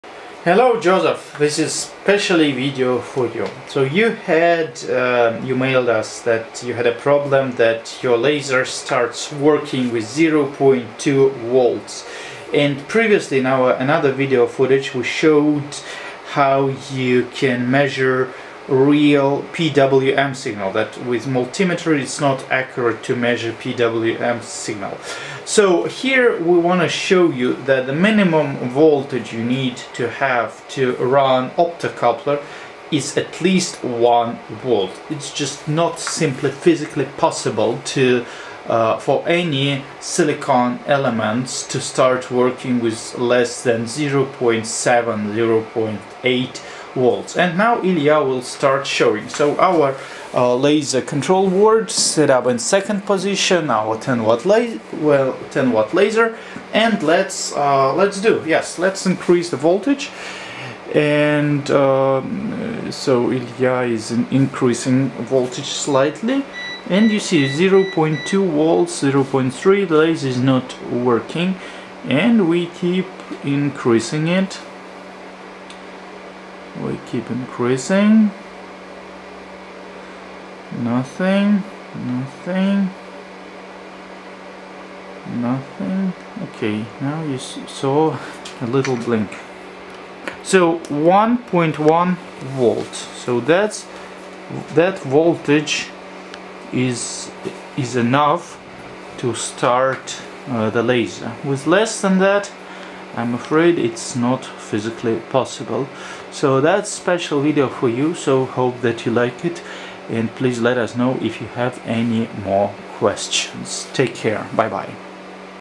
Can you run an Endurance laser with 0.2V signal?! Answer to Joseph (Endurance lasers customer).
A small video showing that Endurance lasers can not be launched with low voltage (less than 1V).

comments,. questions and ideas are welcome!